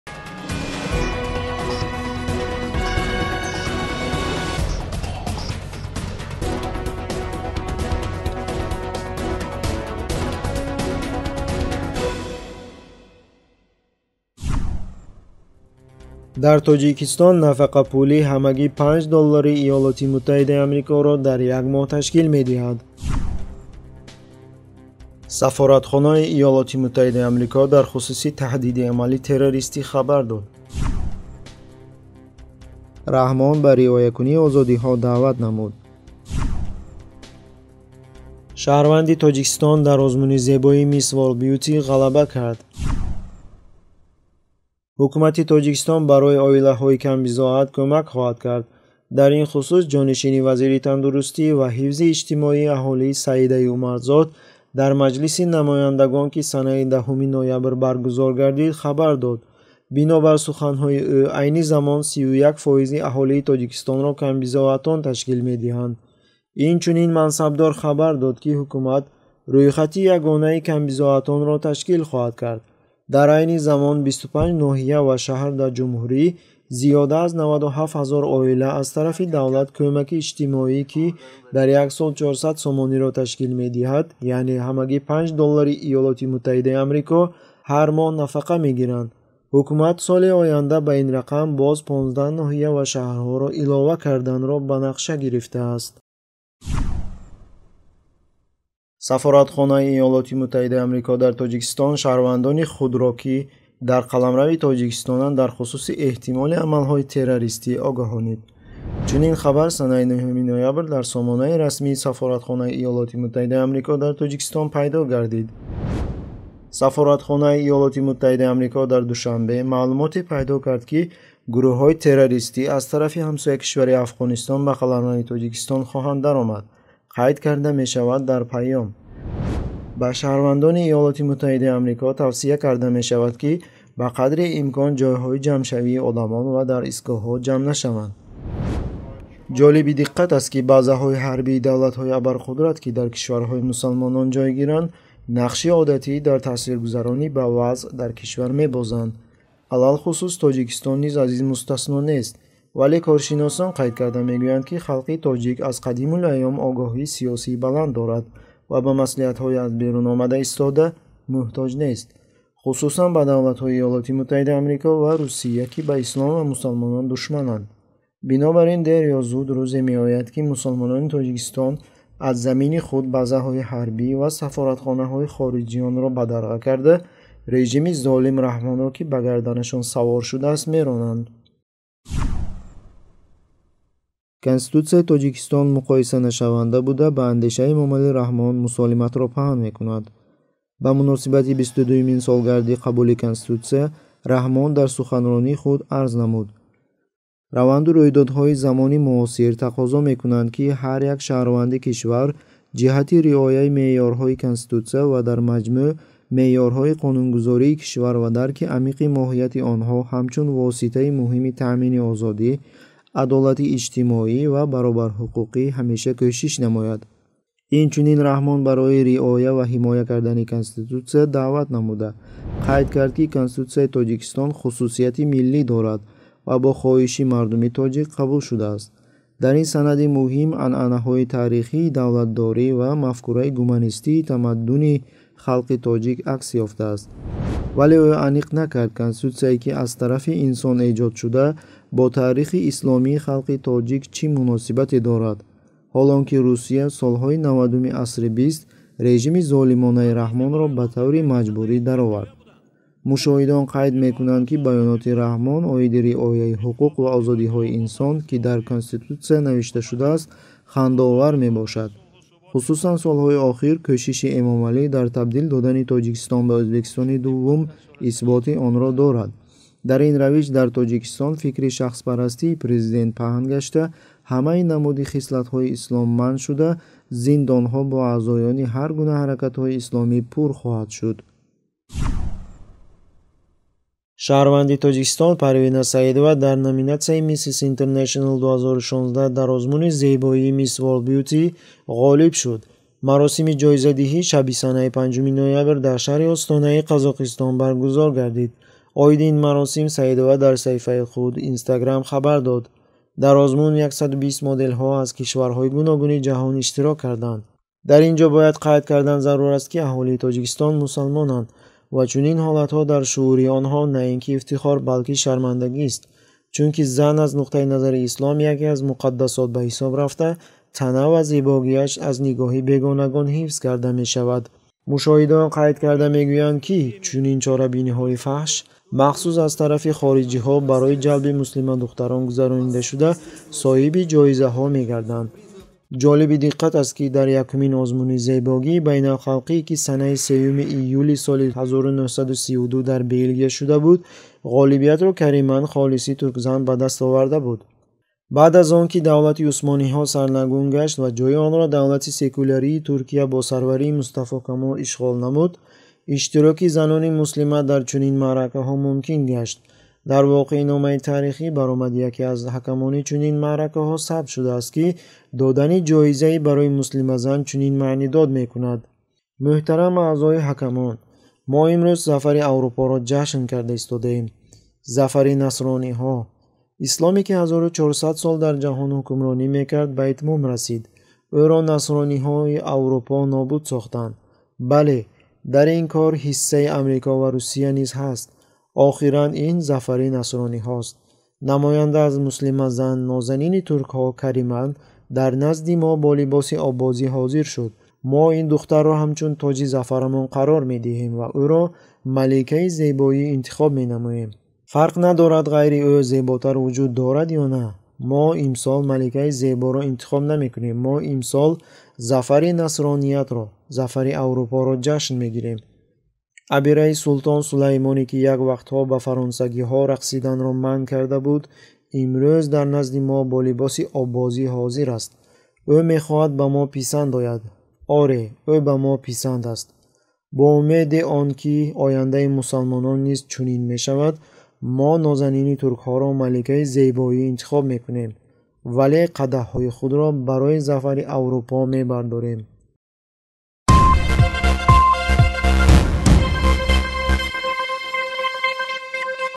Хабарҳои муҳимтарин аз 16.11.2016
1. Дар Тоҷикистон нафақапулӣ ҳамагӣ 5 доллари ИМА-ро дар як моҳ ташкил медиҳад
2. Сафоратхонаи ИМА дар хусуси таҳдиди амали террористӣ хабар дод
3. Раҳмон ба риоякунии озодиҳо даъват намуд
4. Шаҳрванди Тоҷикистон дар озмуни зебоӣ "MISS WORLD BEAUTY" ғалаба кард